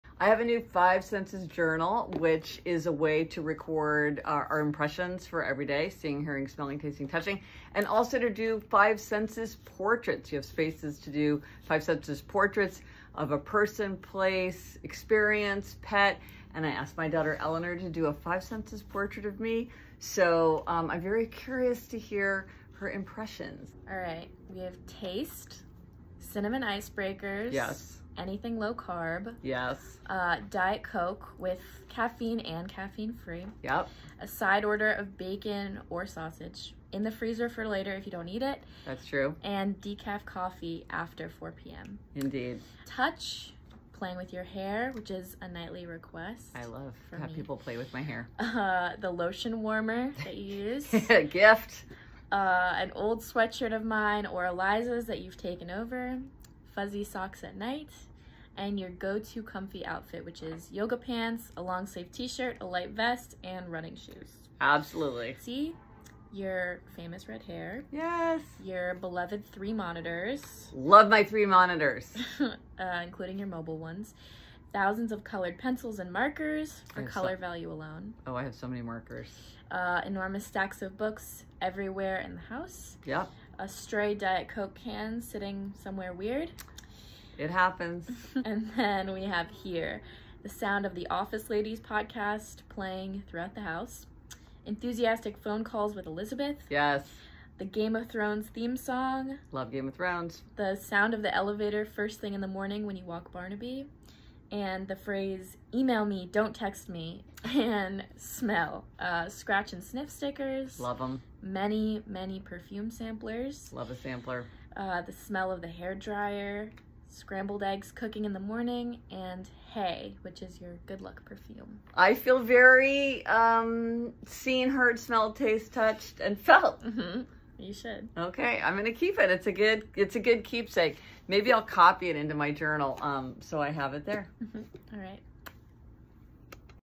My Daughter's Revealing Impressions of Me
Most of my daughter’s impressions of me weren’t a surprise, but a few did catch me off guard.